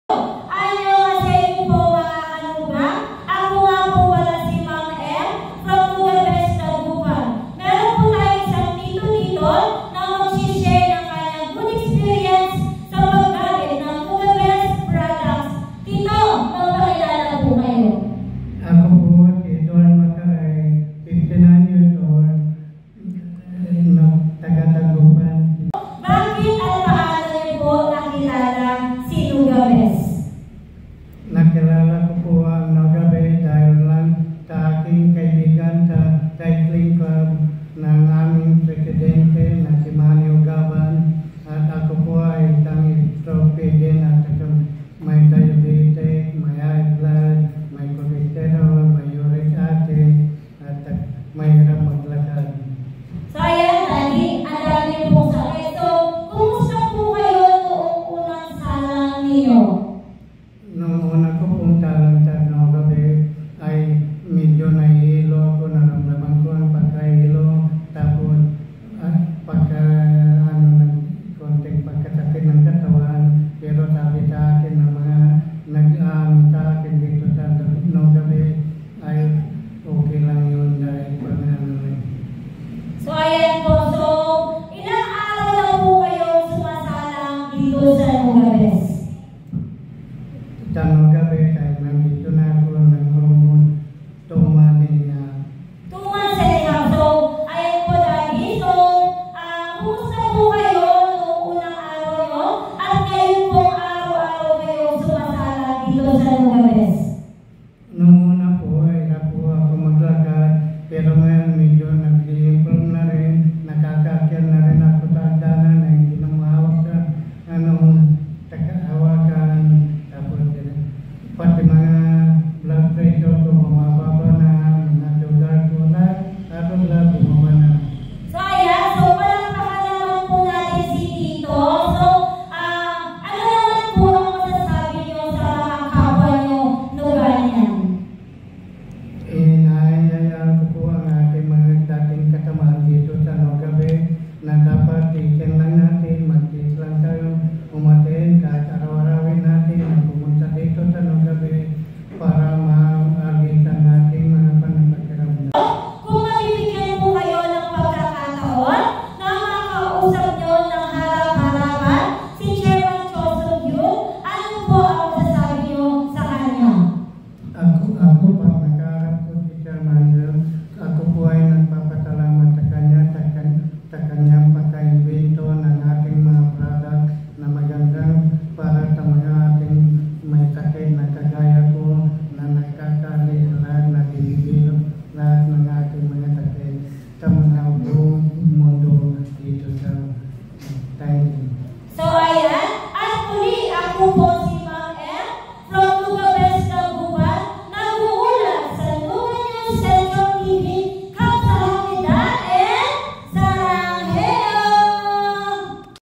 NUGA CENTRAL NEWS JULY 25,2024 #1 DAGUPAN
DAGUPAN BEST HOUSE
Edward Macaraeg
59 years old

Sharing her good improvements using Nuga Best Products

Mild Stroke
High Uric Acid